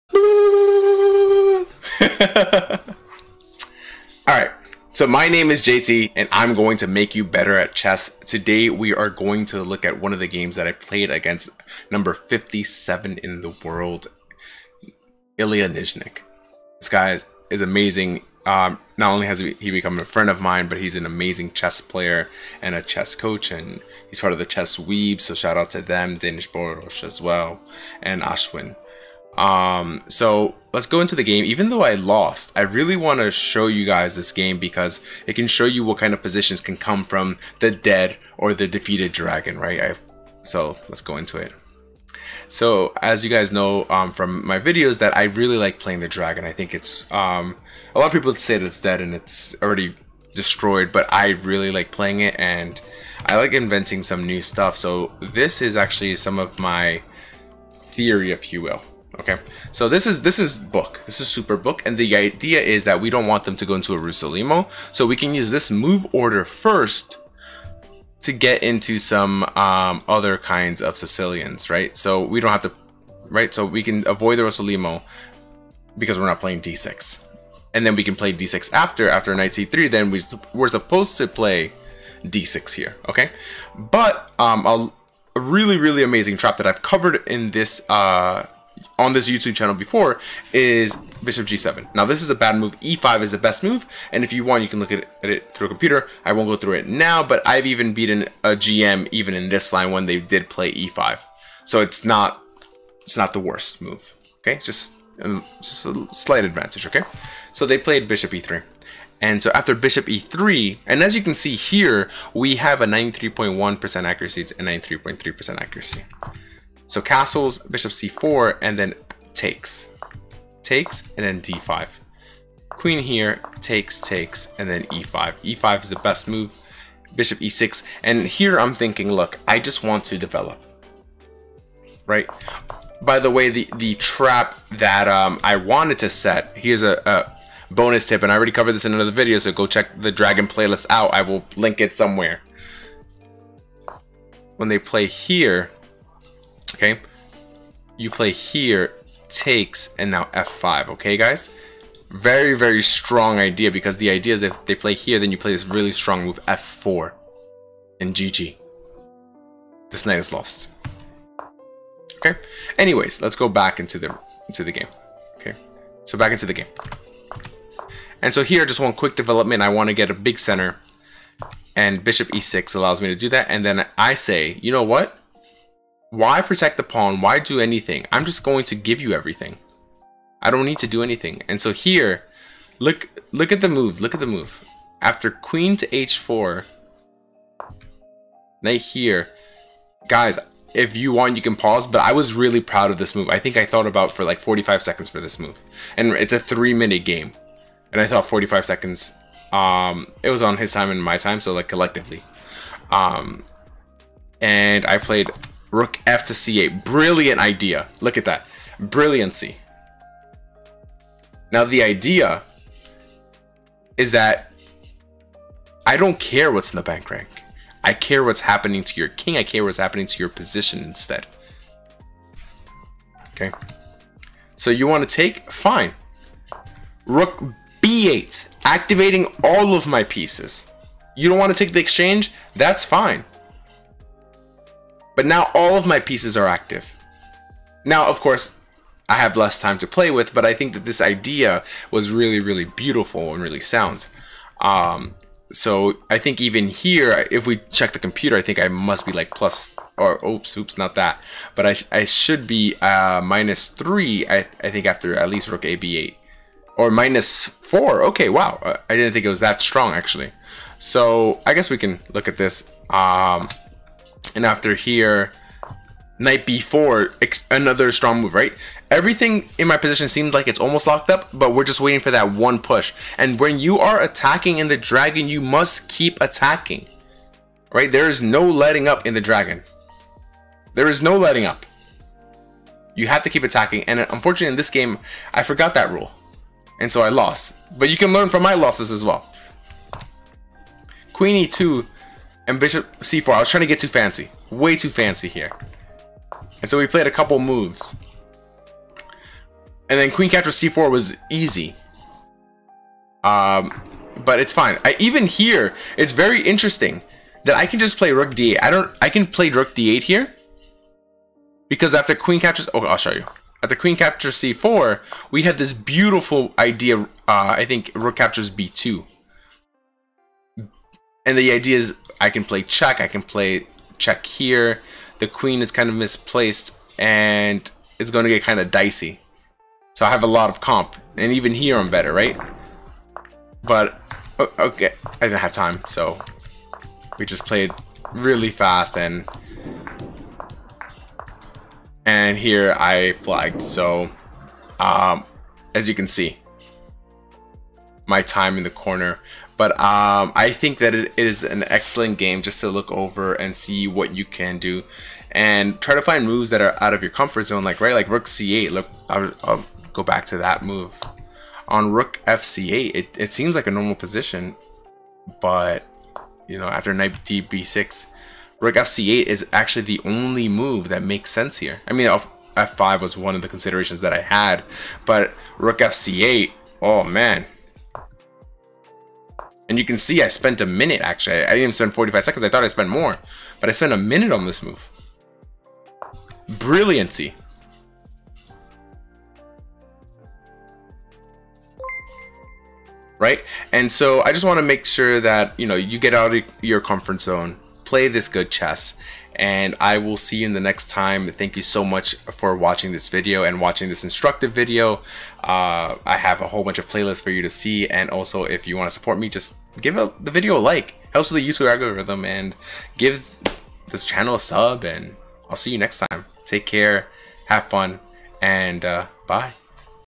Losing against Legends - Illya Nyzhnyk
My game versus #54 in the world Illya Nyzhnyk!
🔥🔥EXPAND THIS 🔥🔥

Books to get from 0-1100

Bitcoin(BTC): 3ECSGN6DeuzRKdRpdvQrxwCfd1aSzpeoQ2

My Links:
Twitch.tv/Japanesetutor